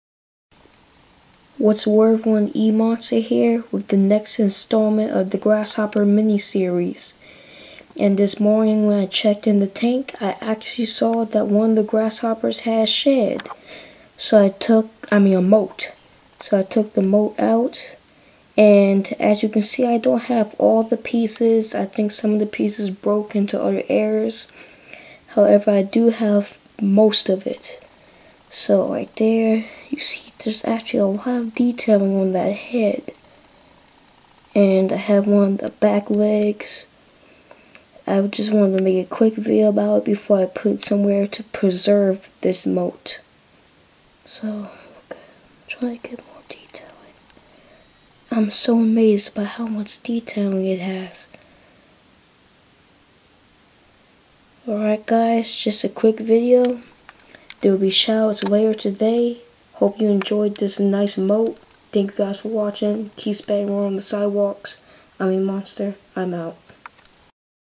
Grasshopper Molt
This morning one of the hoppers had a molt. The molt had a lot of detailing.